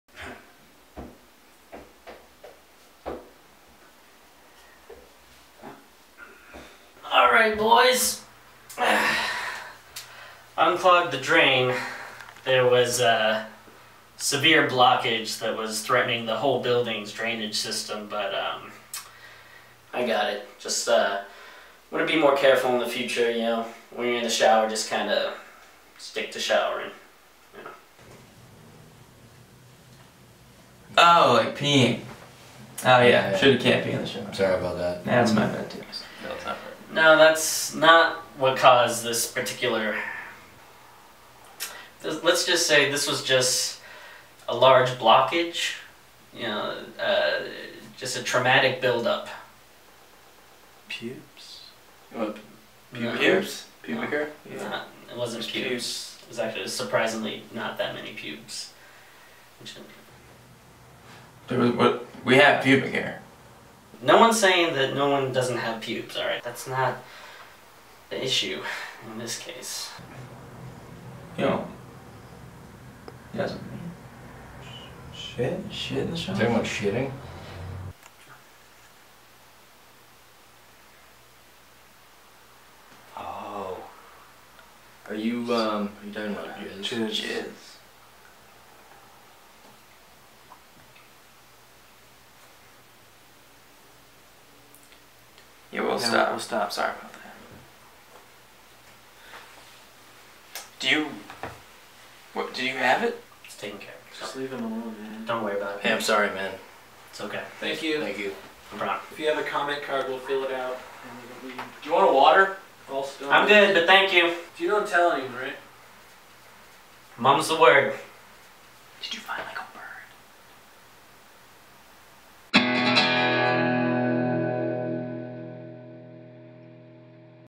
Clogged Pipes
The plumber pays a visit.

Sketch #92

NEW SKETCH EVERY MONDAY

Dead Kevin Sketch starring Ahmed Bharoocha, Ryan O'Flanagan and Jack Robichaud.

Los Angeles based sketch group